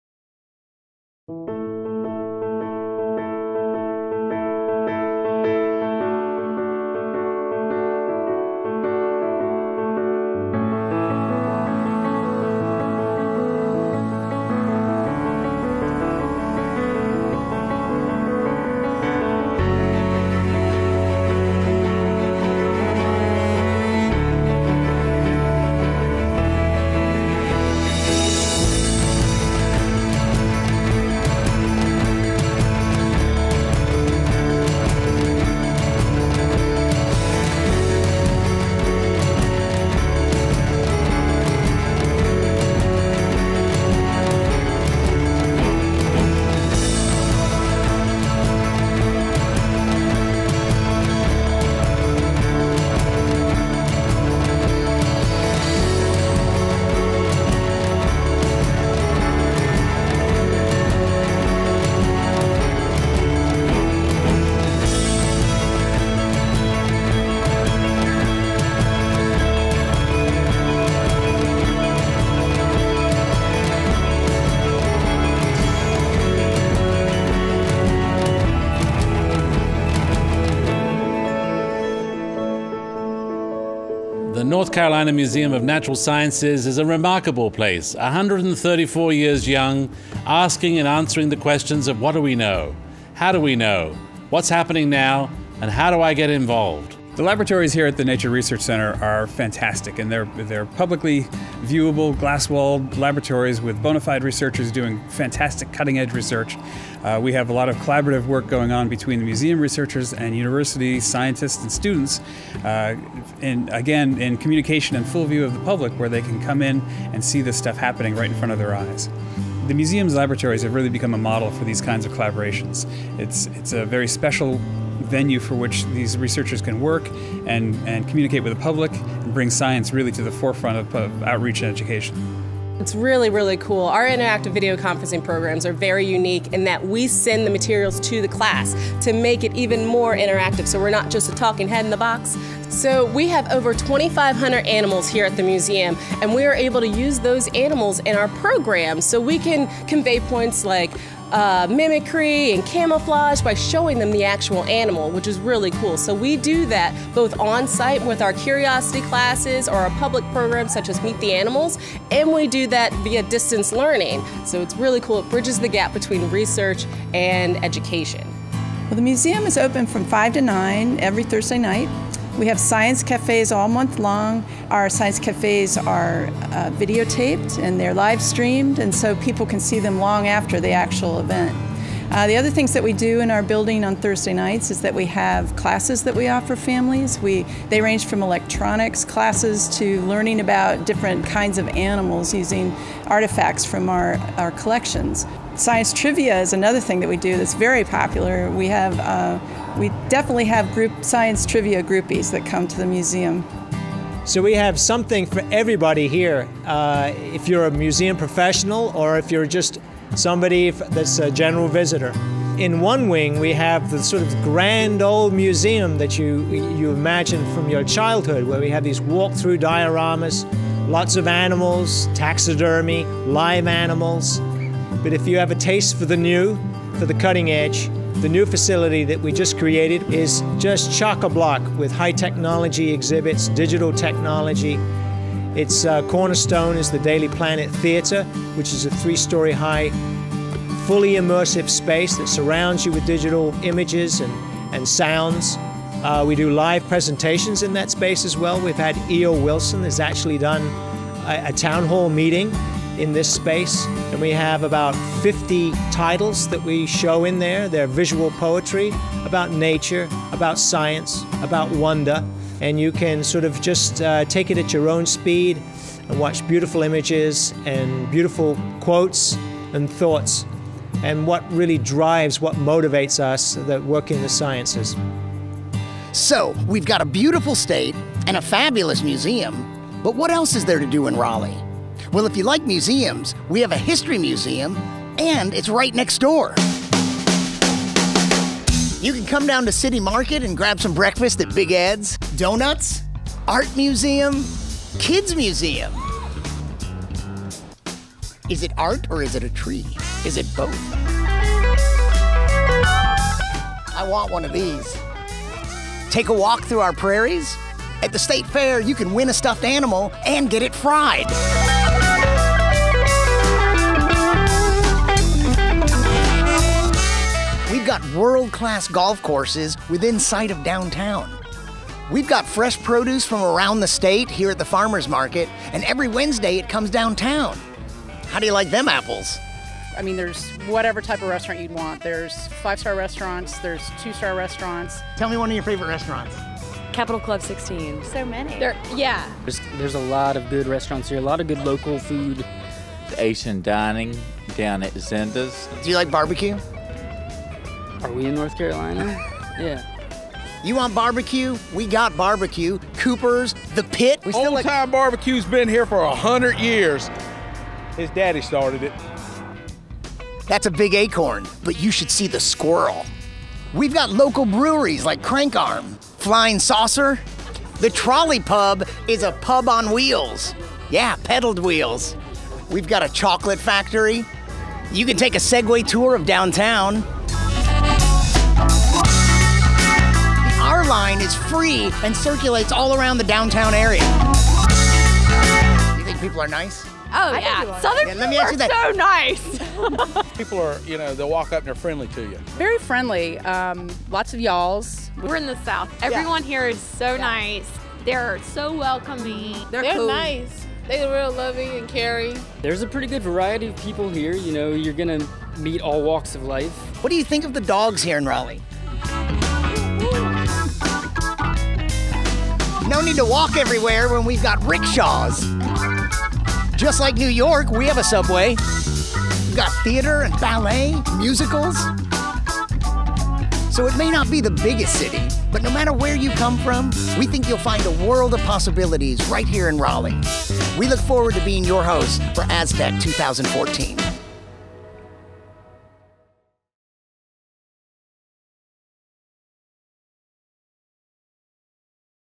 ASTC 2014 is in Raleigh, NC
The annual conference of the Association of Science-Technology Centers (ASTC) will be in Raleigh, NC in 2014. Here is a peek at what is in store for you when you arrive.